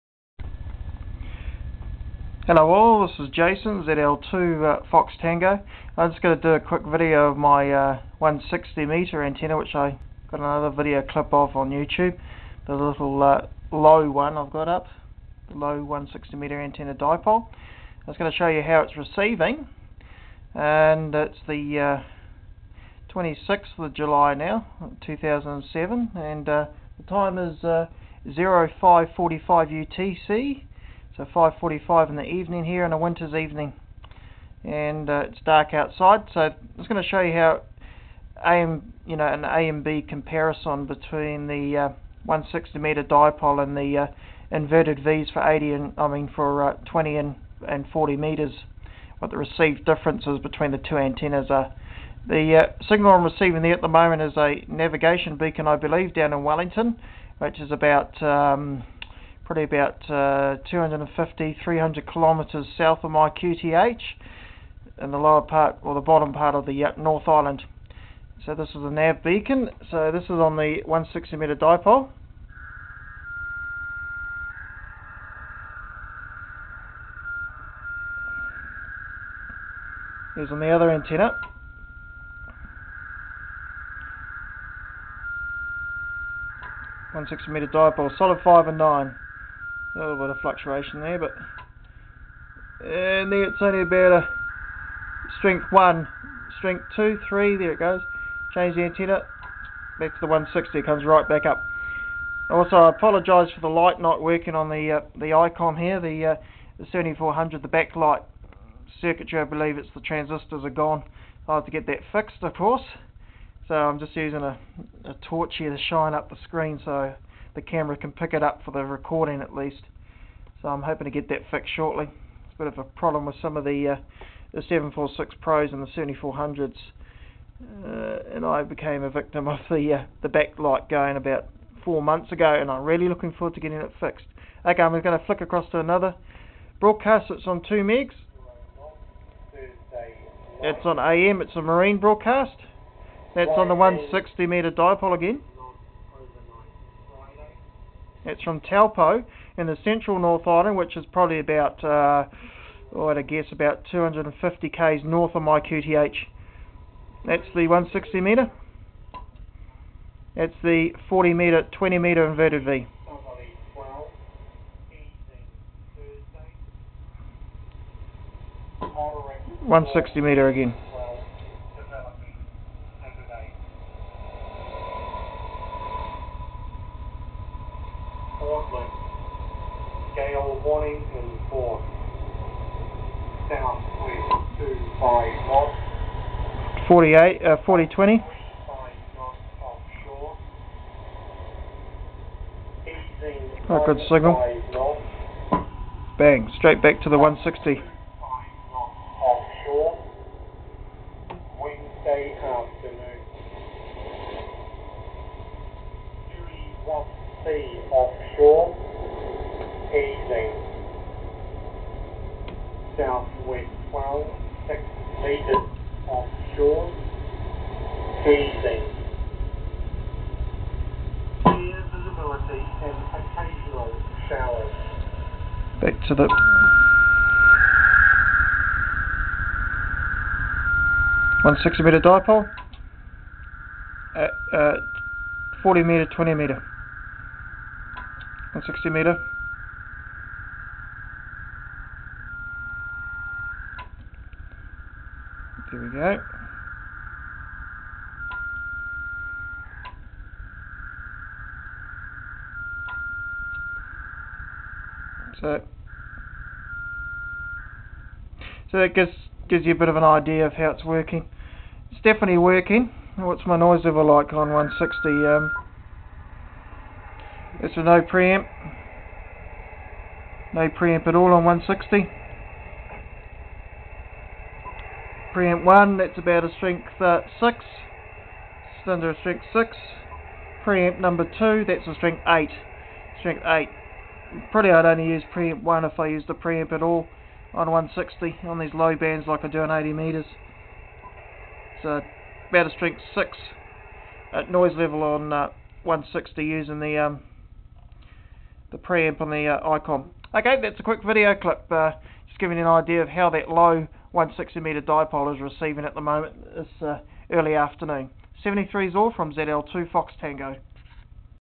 ZL2FT 160Mtr Antenna RX Test
An A and B comparison test on my new 160 mtr dipole antenna.
Testing between the 40/20mtr inverted vee up 30ft and 160mtr dipole antenna on fence.
Stations used to rx during test where on 1.820mhz(from Wellington NZ),and 2.210mhz (from Taupo NZ).